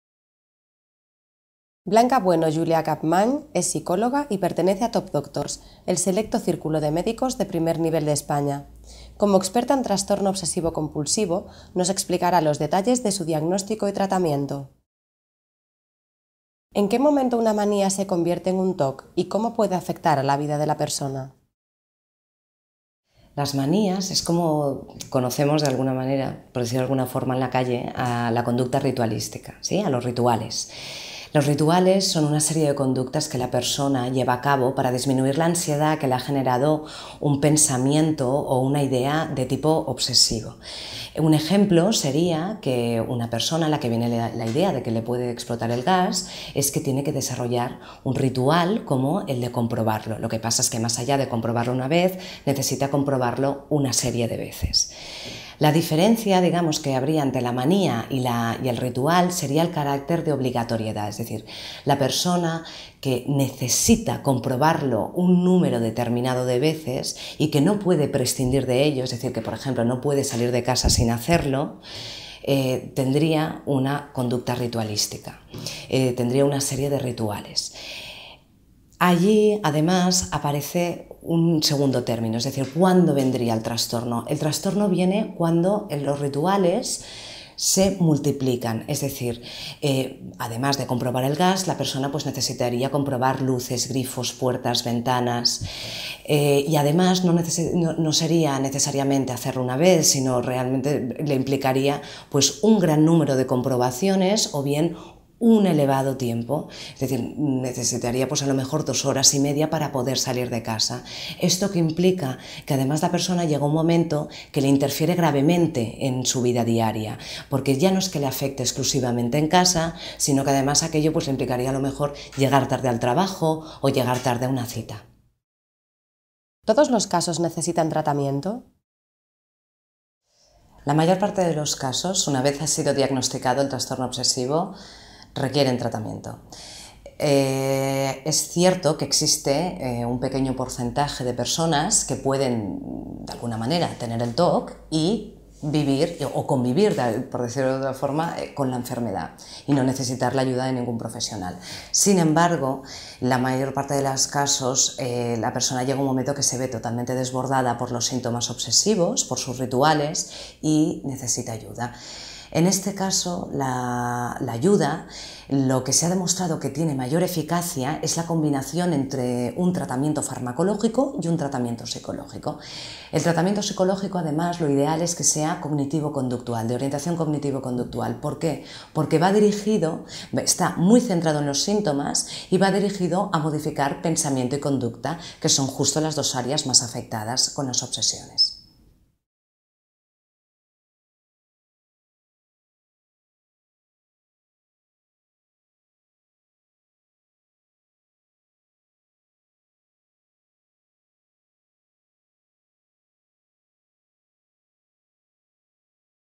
Blanca Bueno Julià-Capmany - Trastorno obsesivo-compulsivo: causas
Blanca Bueno es una psicóloga de primer nivel experta en el tratamiento del Trastorno Obsesivo-Compulsivo. Visita su perfil completo y pide cita

El Trastorno Obsesivo-Compulsivo aparece cuando una persona siente la obligación de repetir una conducta obsesiva para calmar su ansiedad. Esto interfiere gravemente en su vida diaria y es necesario recurrir a tratamiento.